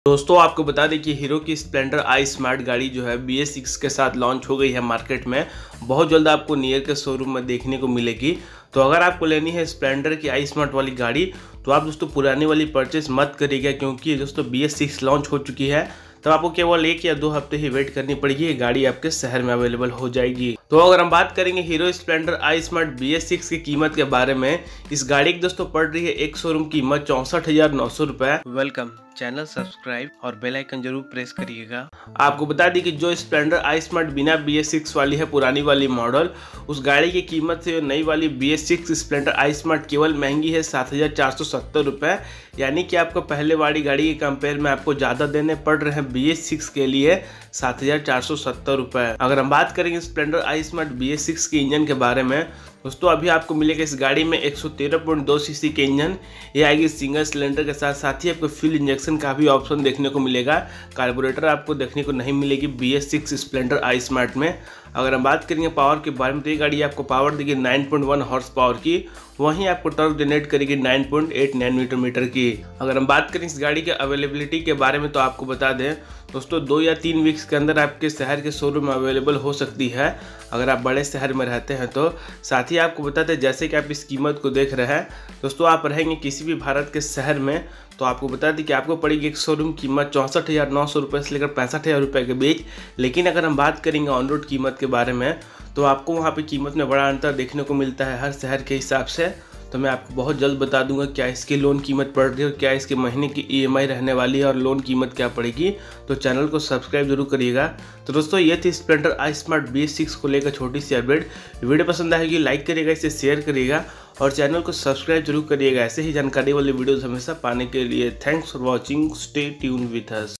अब होगा जलसा Hero Splendor ismart bs6 launched at Best Price in india 2019-2020
Hi..This video about Hero Splendor ismart bs6 ye bike ab launch ho gai hai india me bahut hi shandaar kimat me almost 65000 exshowroom price.also explain Hero Splendor ismart bs6 price & engine and torque.

Queries Solved:
1)Hero Splendor ismart bs6 Price and specificatios
2)Bs6 splendor bike launch date

CONTENT SOURCE OFFICIAL- OWN and Heromotocorp.com